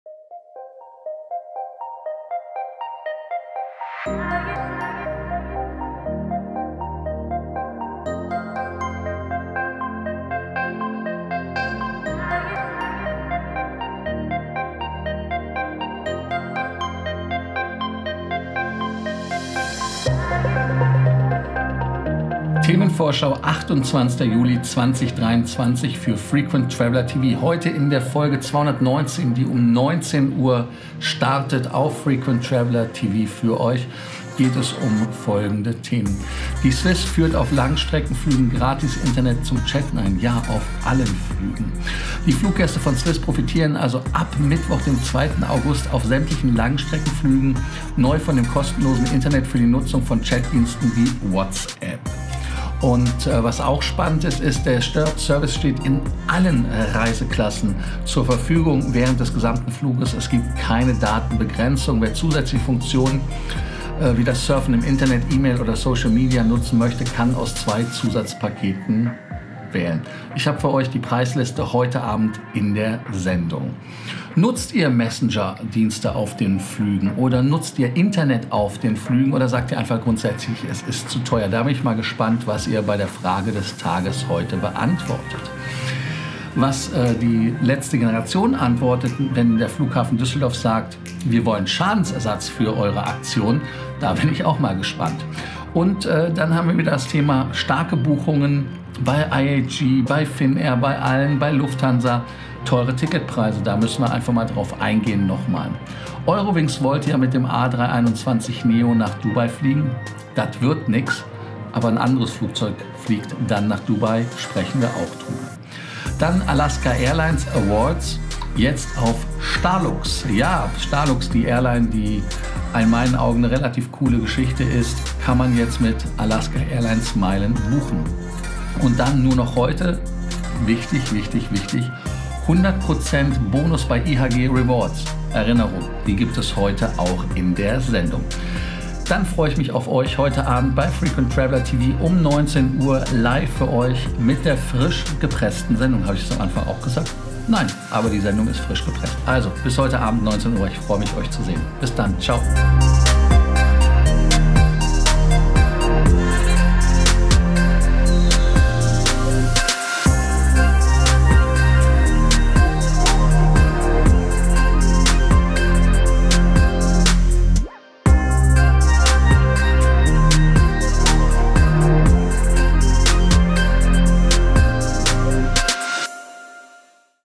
Themenvorschau: 🌐 SWISS führt auf allen Langstreckenflügen Gratis-Internet zum Chatten ein ✈️
Die Themenvorschau auf die heutige Sendung um 19 Uhr deutscher Zeit.

Clubhouse - @LFCorsten